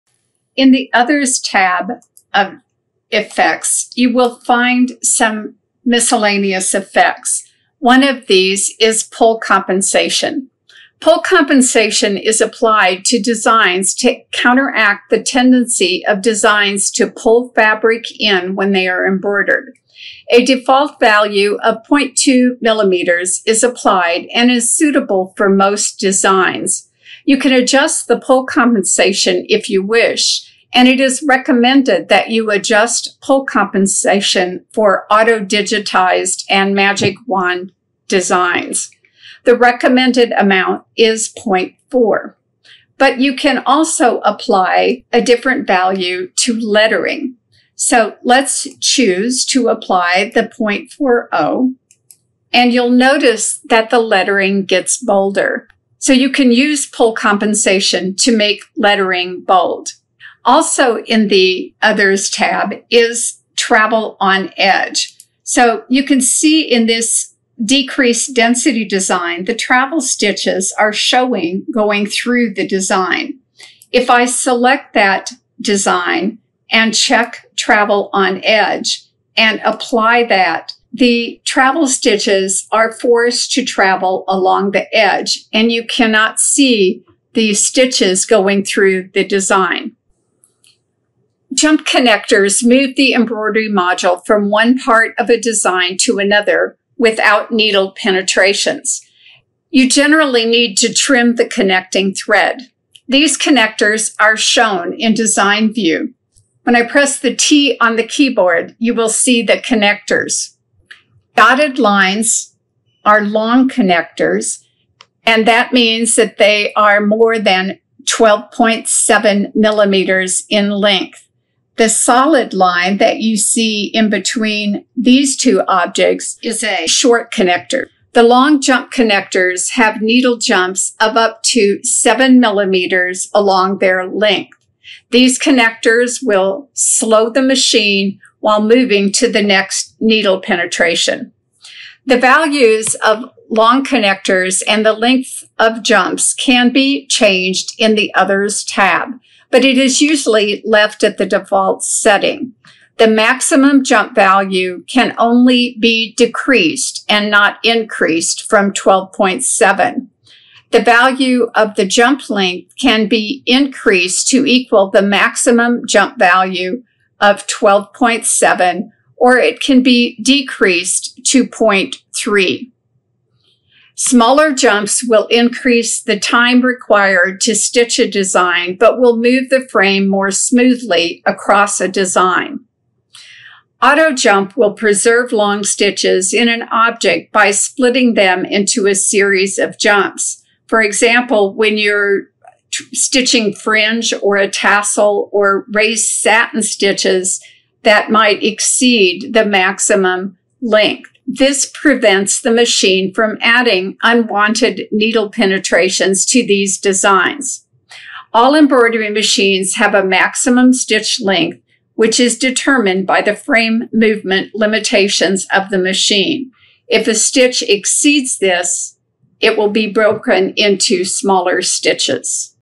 8.10 Master BERNINA Embroidery Software 8 –Stitch Effects: Other Effects
Learn about Pull Compensation, Travel on Edge, and Auto Jump.